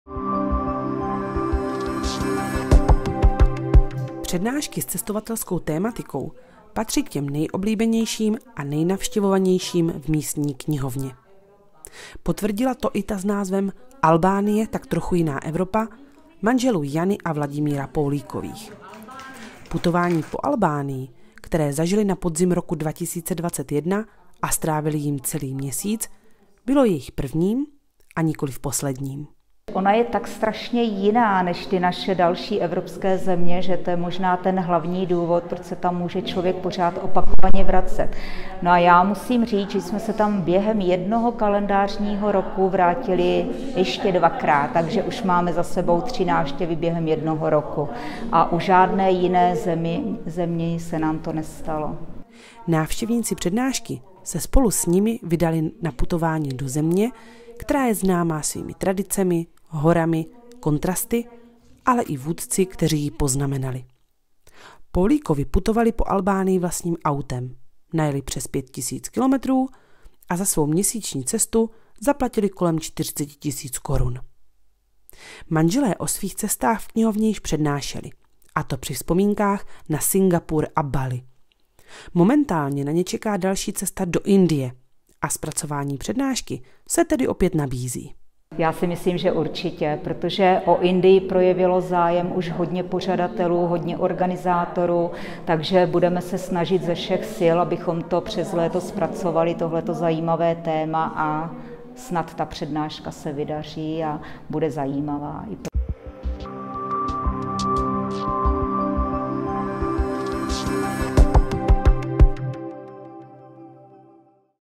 O krásách a kouzlu Albánie s Poulíkovými
Při svém putování po světě navštívili manželé Poulíkovi přes 40 zemí, o jedné z nich povídali návštěvníkům hustopečské knihovny ve čtvrtek 15. února.